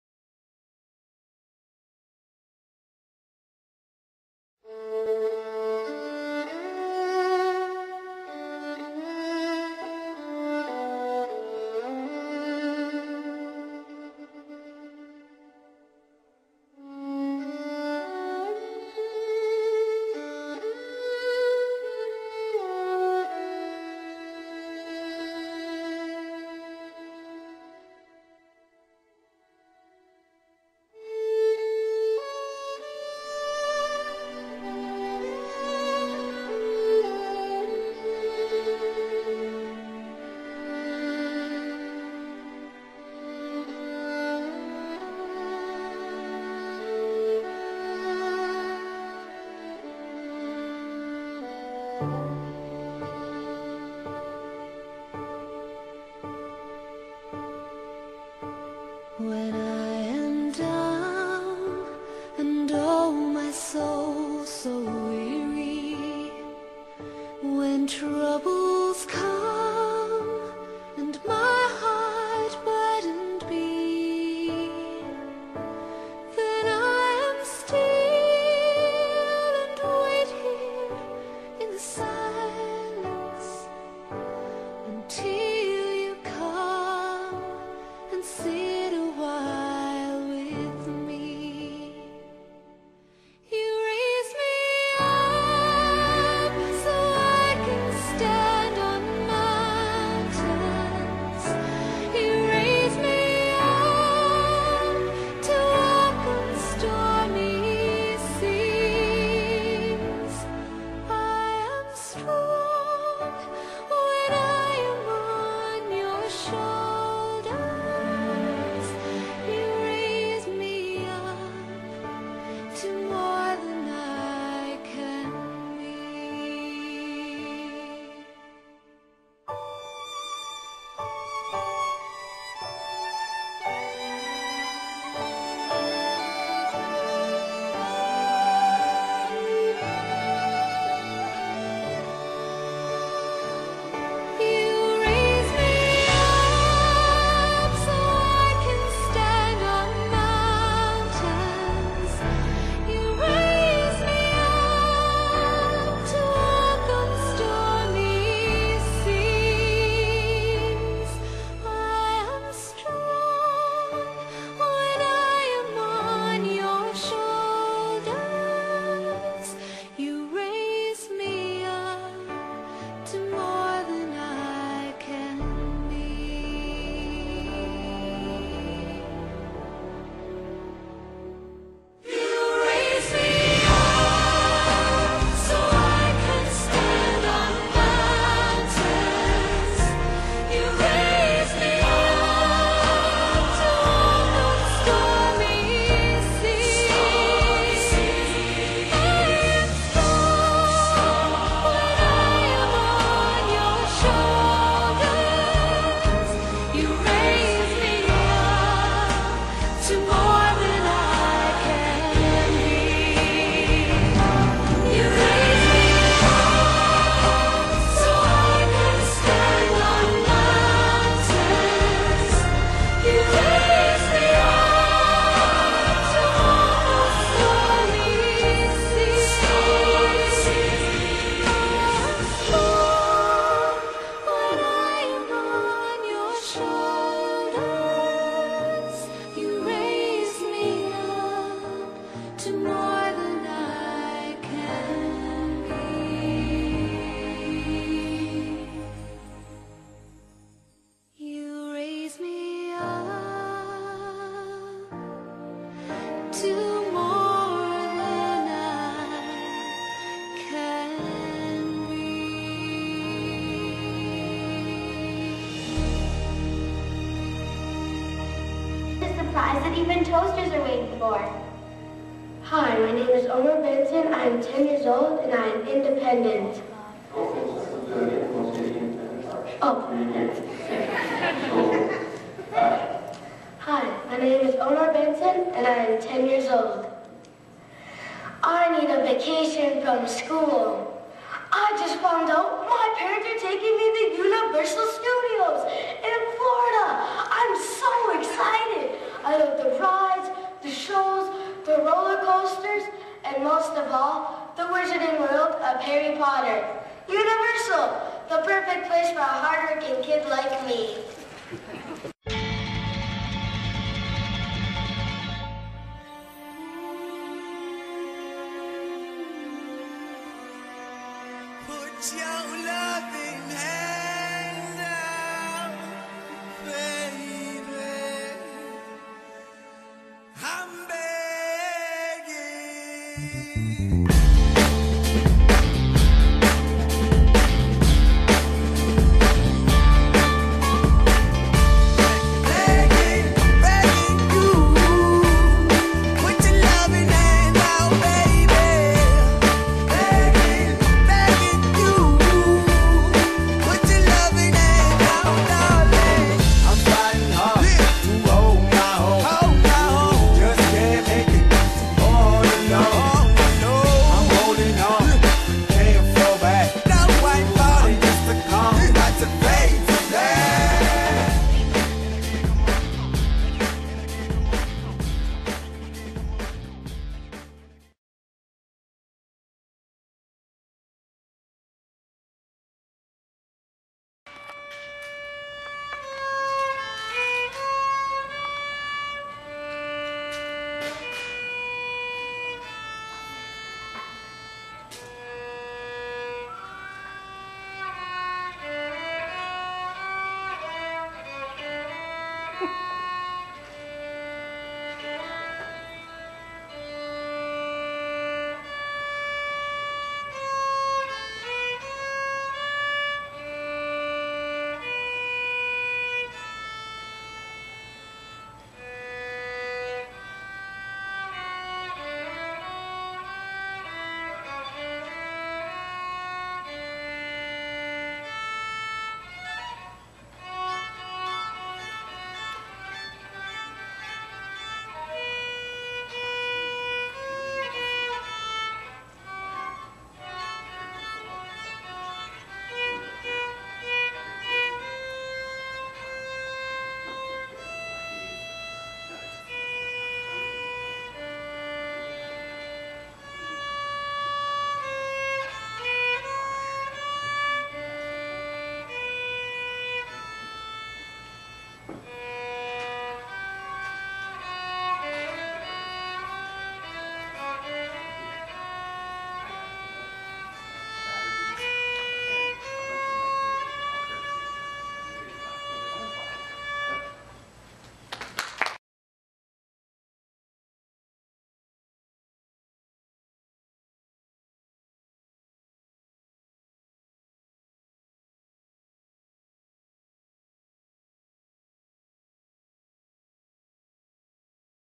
Omar Benson Talent Gold Winner 2012 Talent Inc Canada National Conference Toronto
Amazing Journey with Omar Benson at Talent Inc. Canada 2012
1st year in film, a work in progress. Showing hard work in rehearsals in Film Commercial/Scene, Jazz Dance, Violin, Photo runway. This u-tube video is the winner of 2012 1st National Talent Conference with Talent Inc Canada 2012.
2 years in a row 2013 Gold Winner Talent Inc Canada Conference 2013